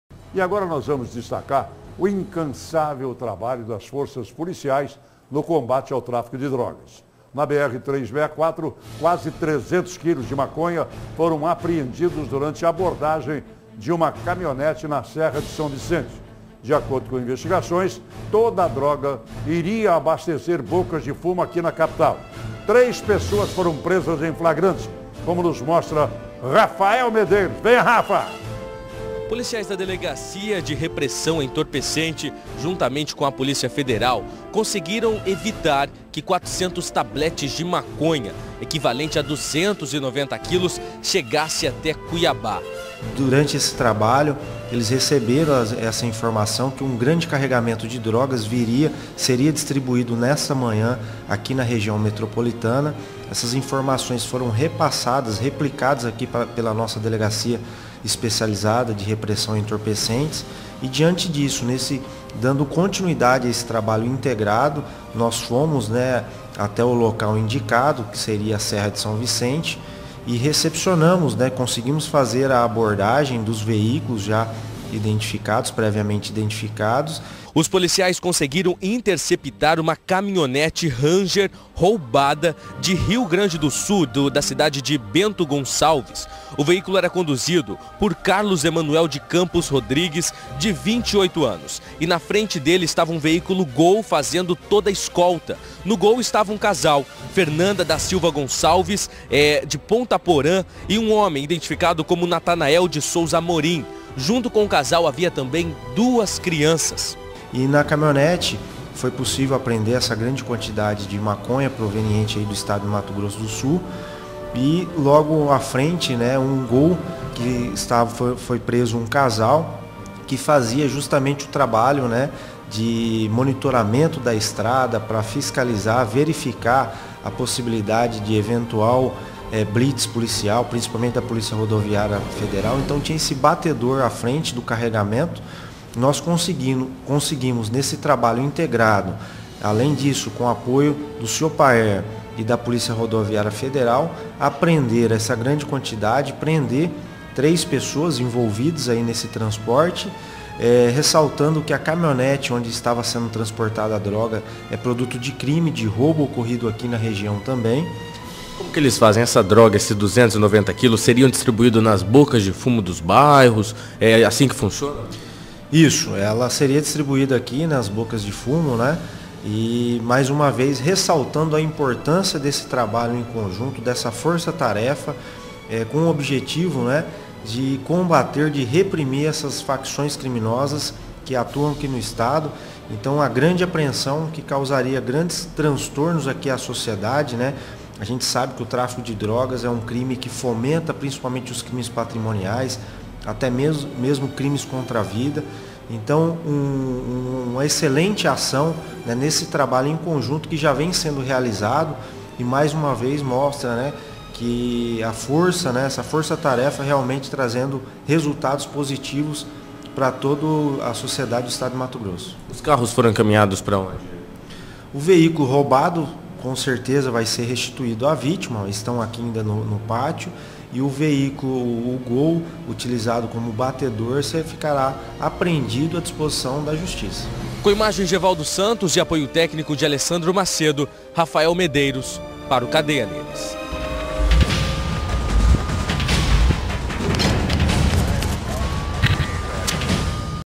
Trio é preso com quase 300 kg de maconha em caminhonete
Segundo as investigações, os entorpecentes seriam usados para abastecer bocas de fumo aqui da Capital.